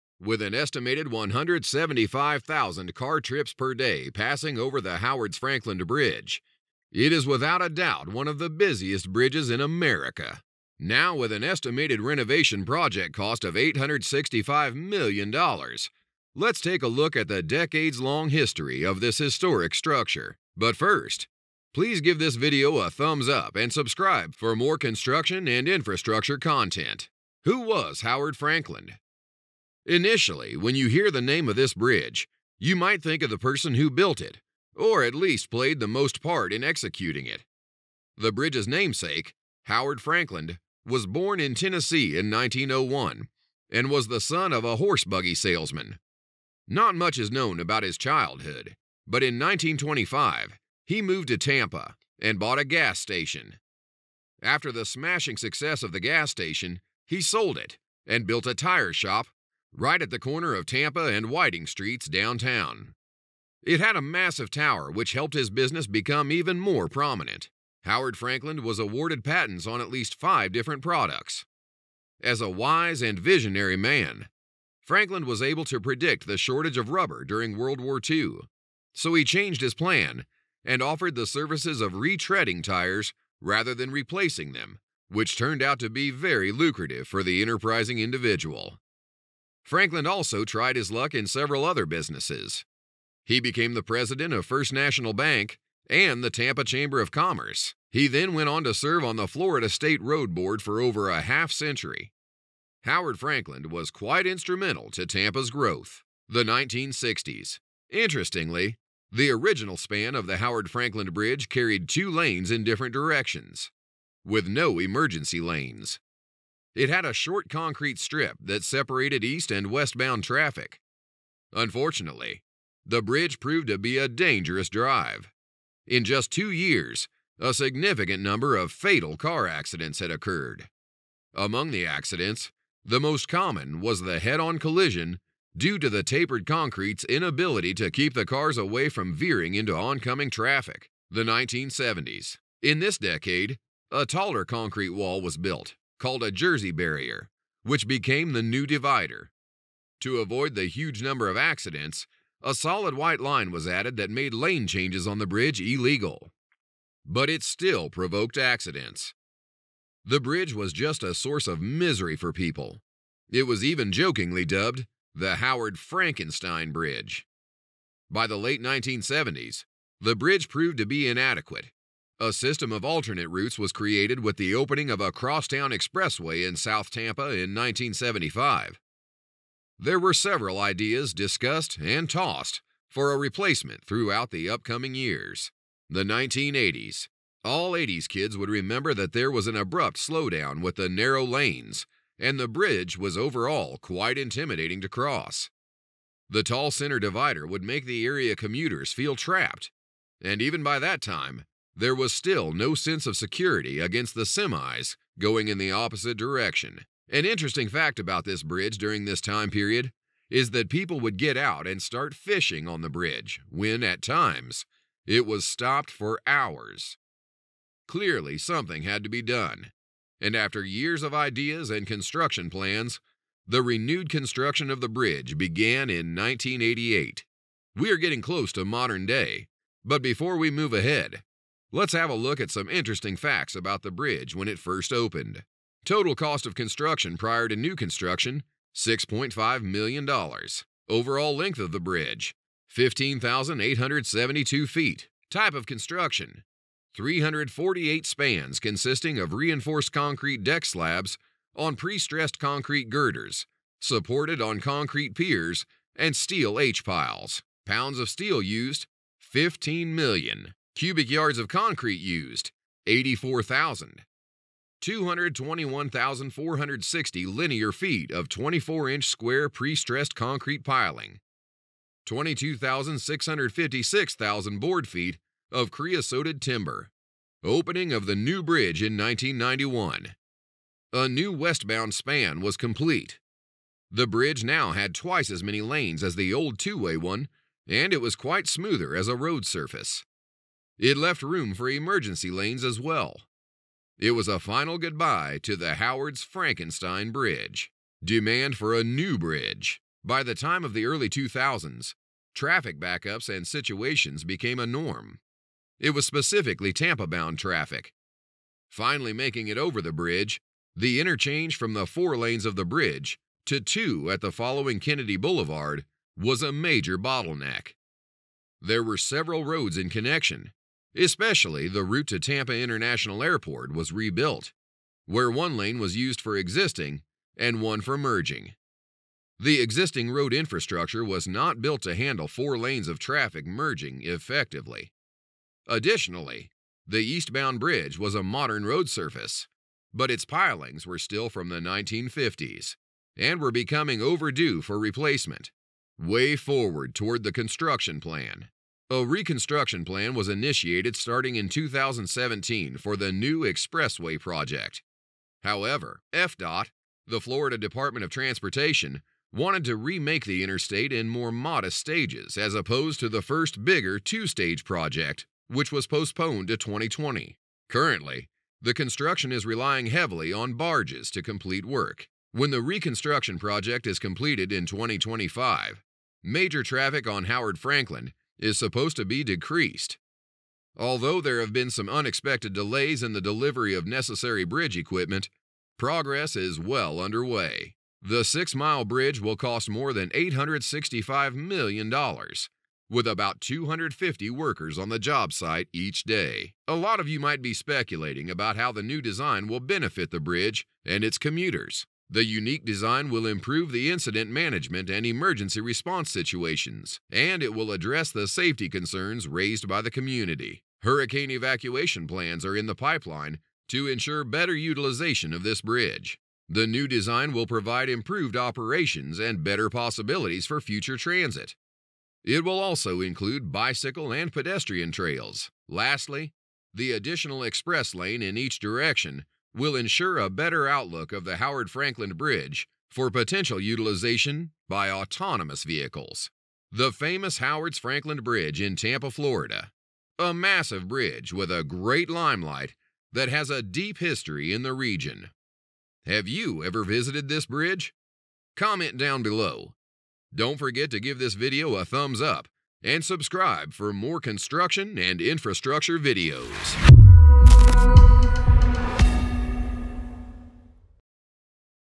Howard Frankland Bridge | History & Facts - Tampa, Florida
With an estimated 175,000 car trips per day passing over the Howard Frankland bridge, it is without a doubt one of the busiest bridges in America. Now with an estimated renovation project cost of $865 million, let's take a look at the decades long history of this historic structure!